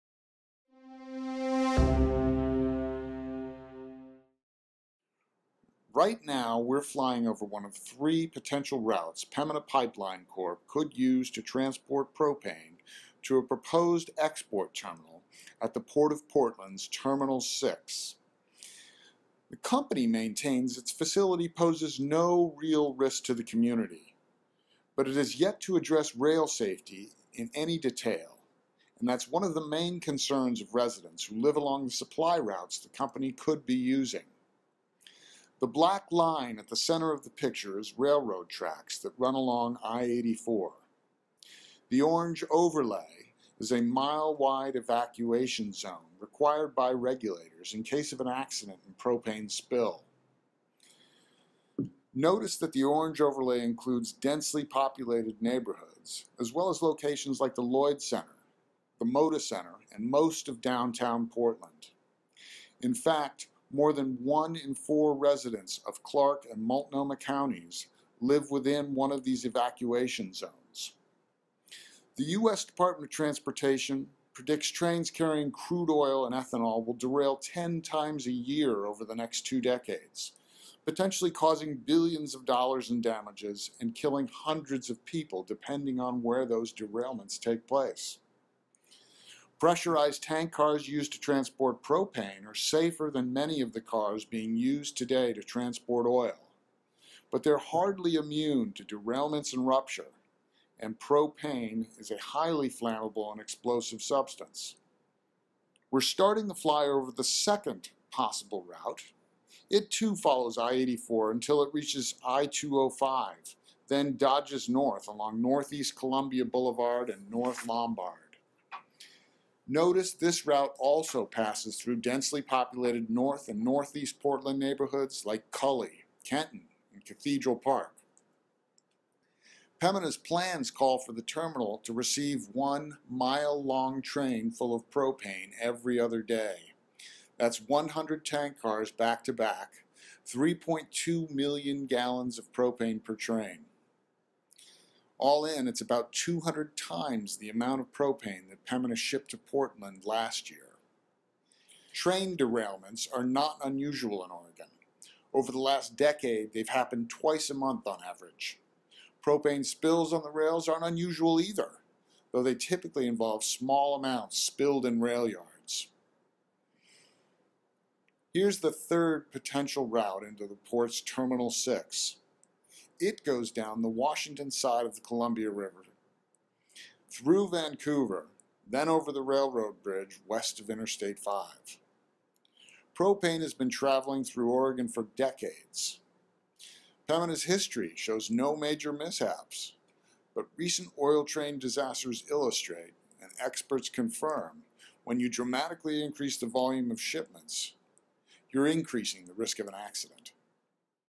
Propane by rail in Portland: A birds eye video of rail routes to proposed export terminal
Rail safety has always been one of the biggest question marks surrounding Pembina Pipeline Corp.'s plan to build a propane export terminal in North Portland. With that in mind, check out this flyover of the routes the propane could take through Portl